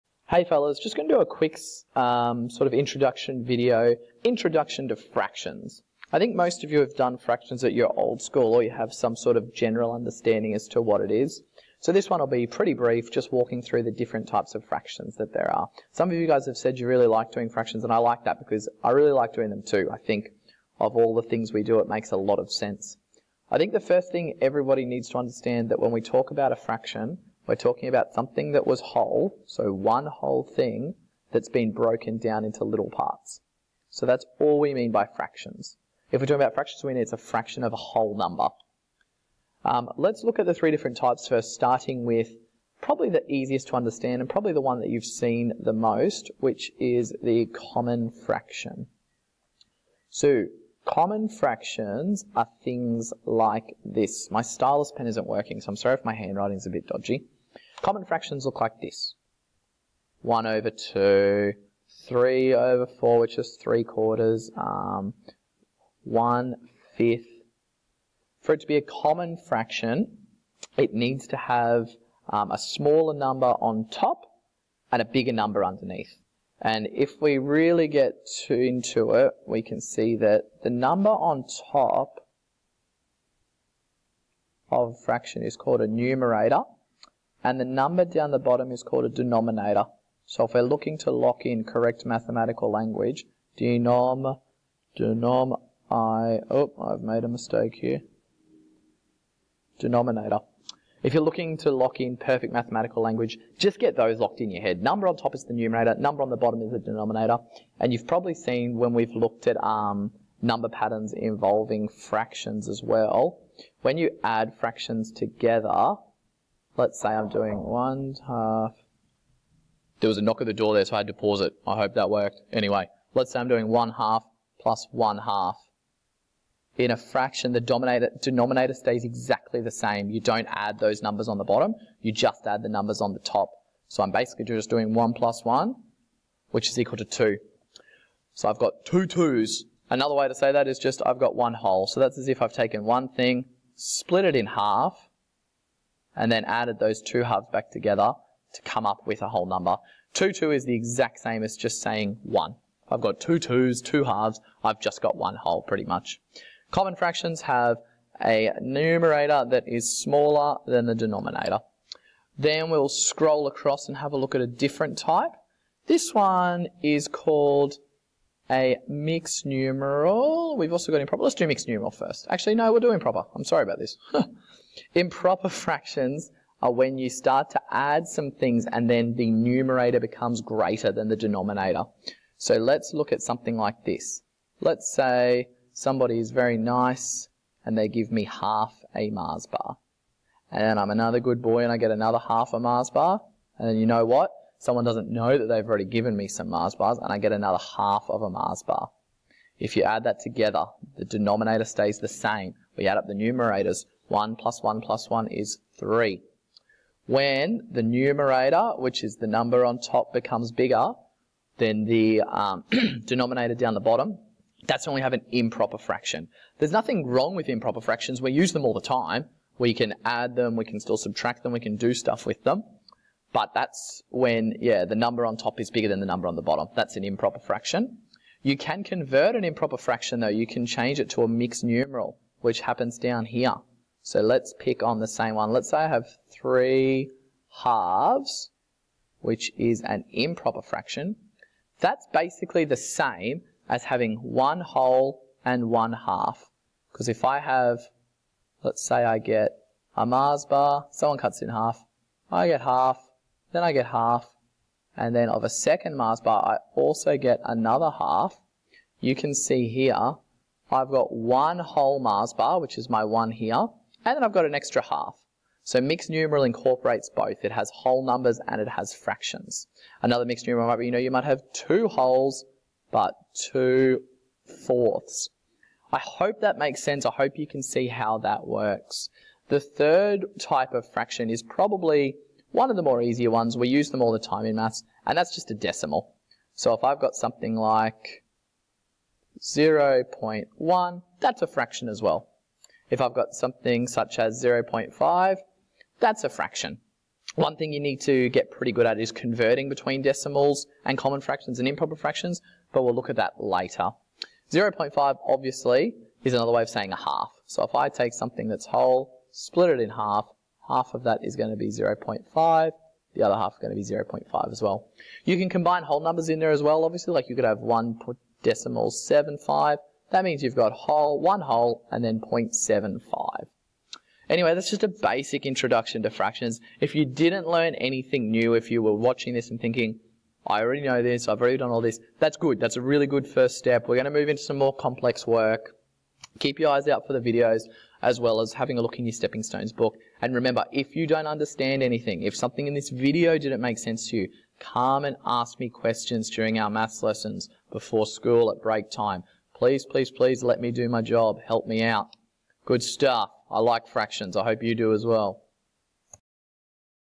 IntroductionToFractions
Links to 6.7, 6.8, 7.1, 7.2, 7.3, 7.4, 7.5 7.6, 7.7.
Basic introduction to the various types of fractions we will be dealing with